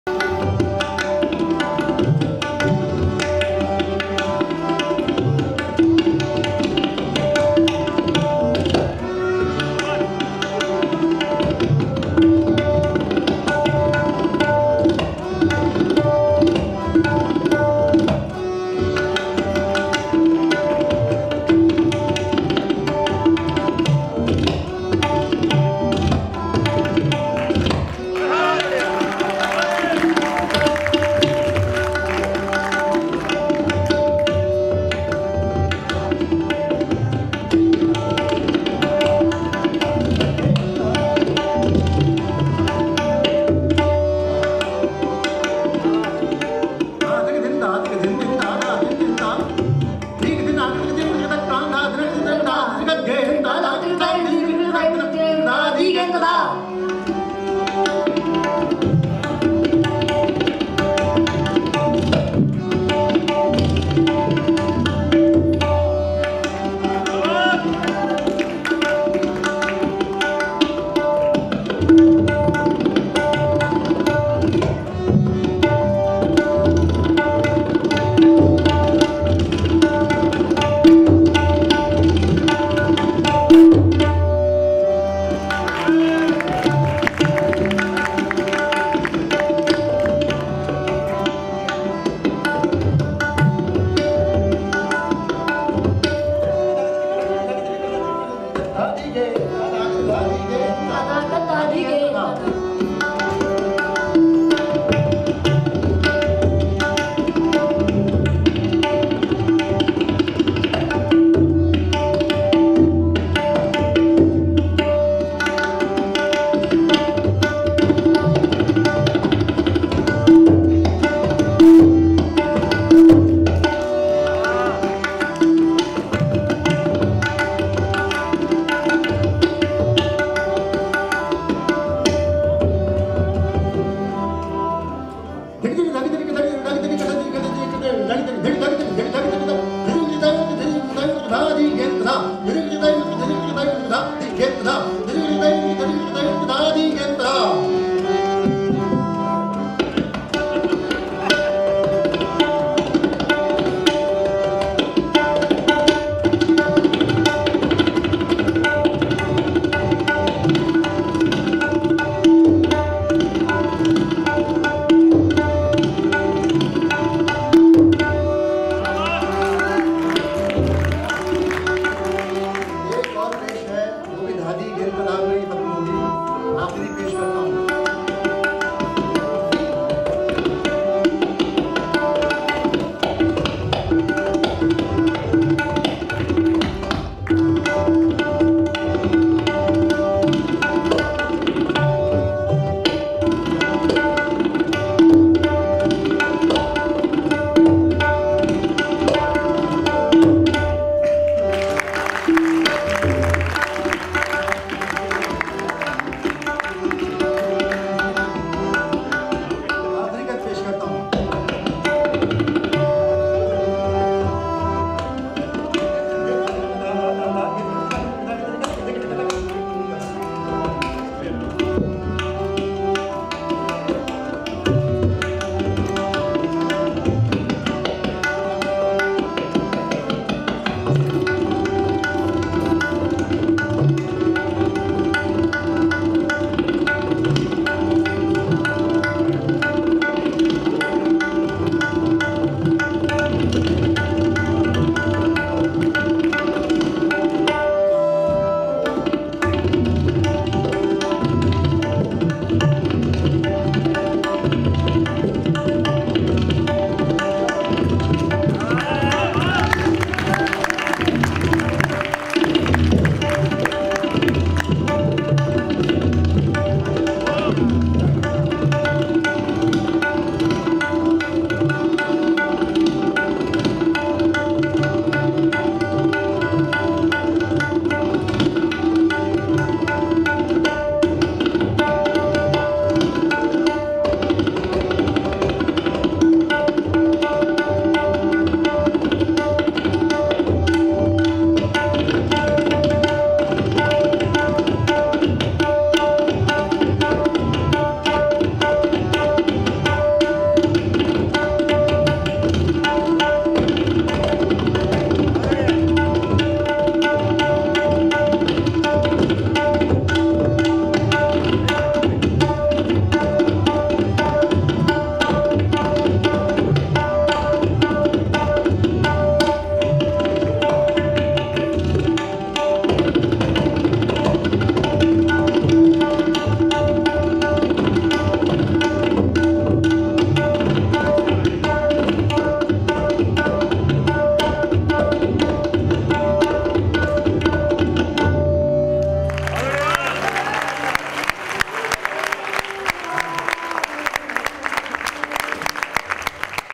Ajrada Gharana Tabla Maestro Ustad Akram Khan and Son Zargham Khan's Mesmerizing Jugalbandi
Ajrada Gharana Tabla Maestro Ustad Akram Khan and Son Zargham Khan's Mesmerizing Jugalbandi. Ustad Akram Khan has been playing tabla for past 43 years. He started learning at the age of eleven and since then he has followed his passion in tabla. He has been attracted to this form of music since early years due to his proximity to tabla maestros from Ajrada Gharana and since then he has dedicated his life in carrying ahead the legacy of the Ajrada Gharana and is now passing on this legacy to younger generation of tabla lovers. His first performance was in Lucknow at the age of 17 years and his first International performance was in Norway in 1987 when he was just 22 years old. Since then he has performed in more than 11000 concerts across the world. His fans are spread across the globe. His concerts are widely appreciated, and he figures in the top-grade artiste list of ICCR representing India at major concerts and exchange programs across the world. He has performed on All India Radio more than 800 times and more than 150 performances on various television channels. He has performed in several charitable functions for social causes and also joined hands with NGO SPIC MACEY for the spread of music in young generation.